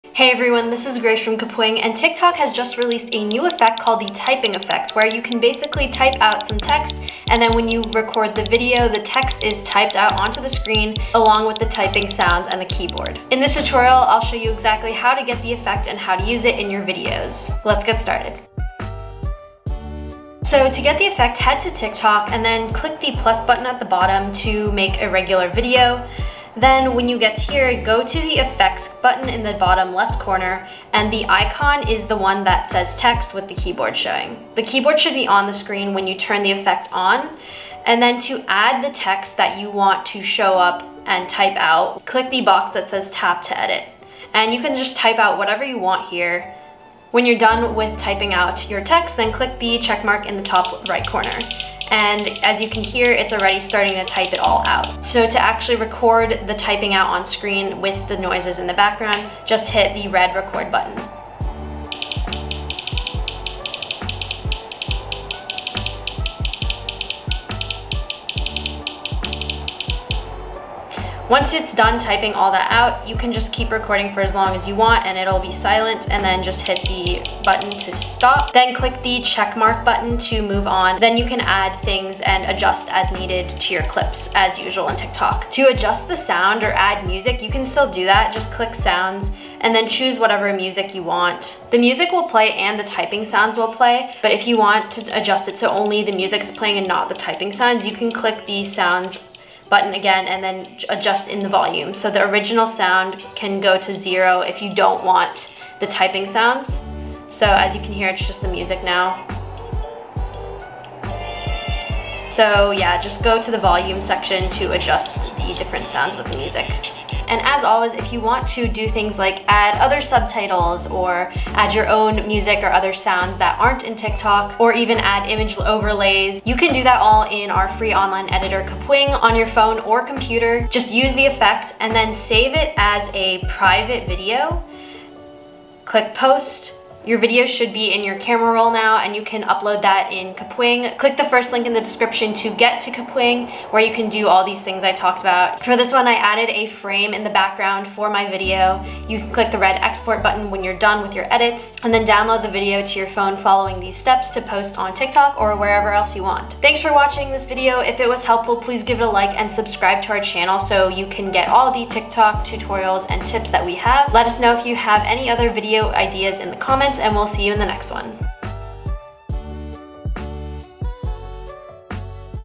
How to Use the Typing Effect on TikTok (Keyboard Typing Sound)
The typing effect is a new TikTok feature that simulates you typing on a phone, where it types what you want on screen while the keyboard is displayed and the typing sounds play in the background. Creators are using it to share confessions and messages they want others to hear, with the text in many videos starting with "I don't know how long I can continue to pretend..." or "to be honest..." In this tutorial, we show you where to get the effect and how to use it, then how to make other edits like adding sounds, text, and image overlays which you can't do in TikTok using an online editor that works on any device called Kapwing.